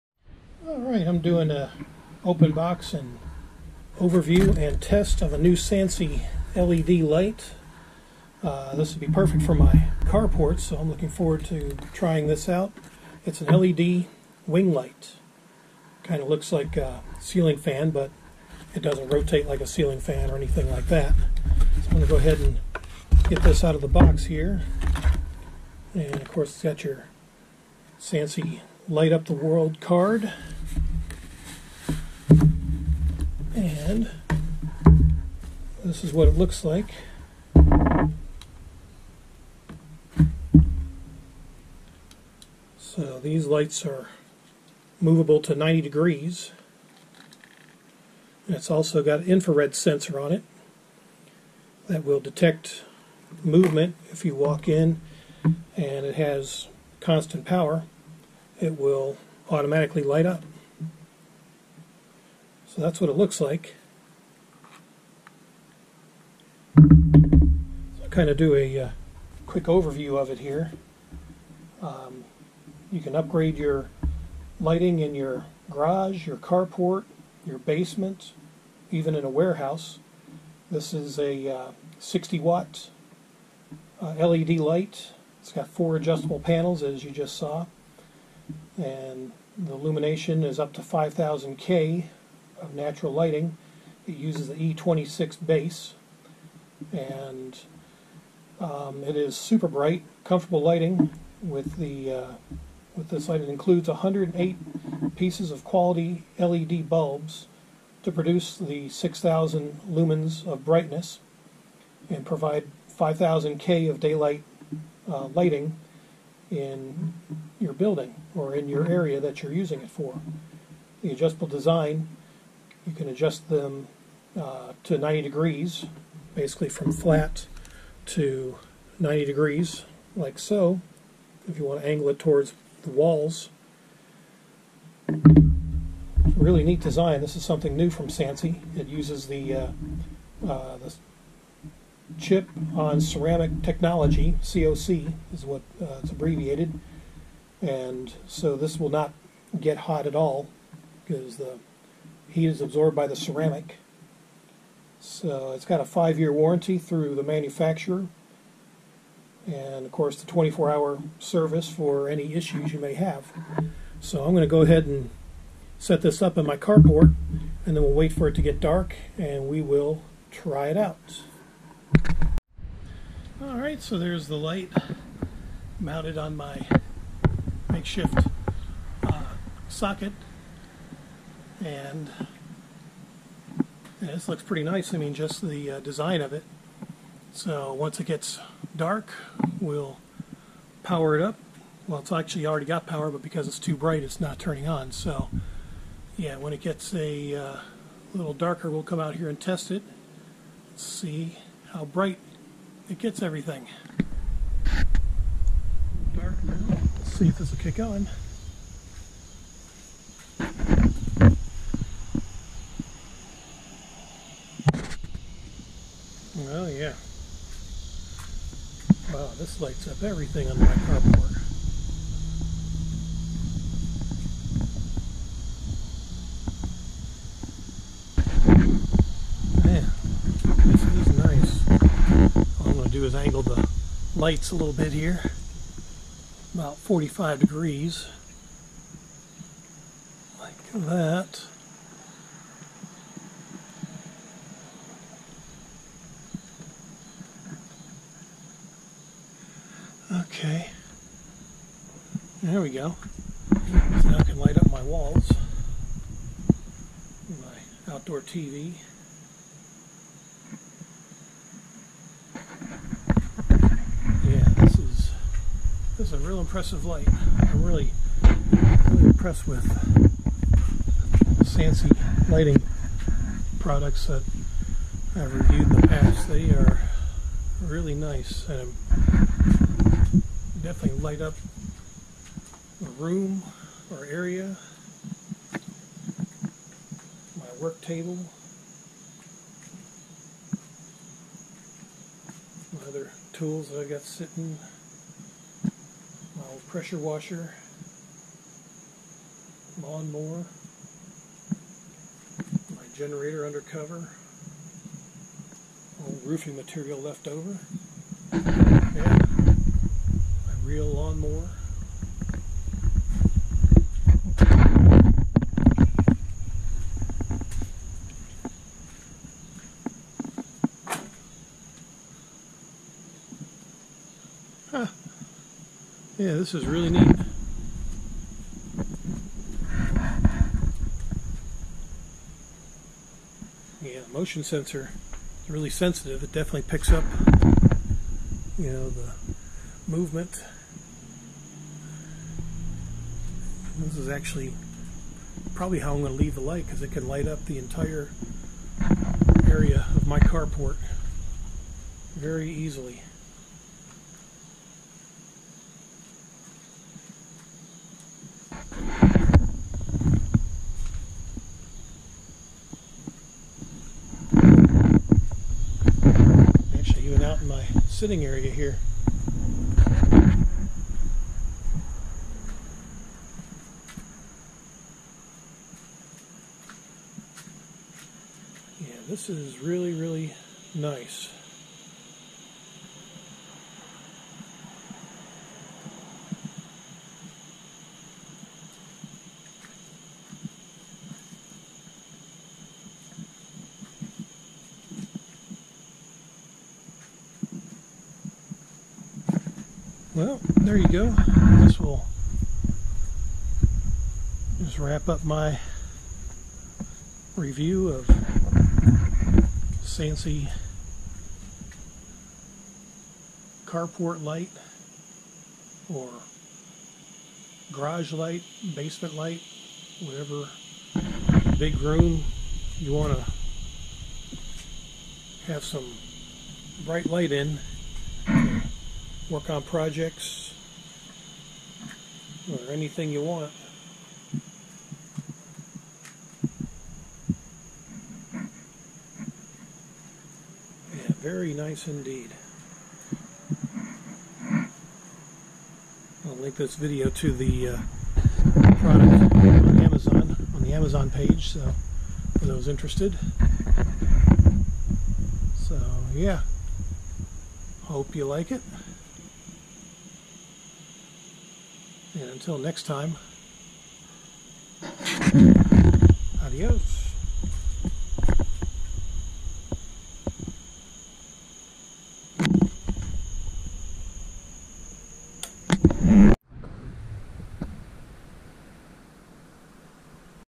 Testing a Sansi LED Carport Light
Another amazing LED light from Sansi!

Save an extra 40% when you apply the coupon on the product page.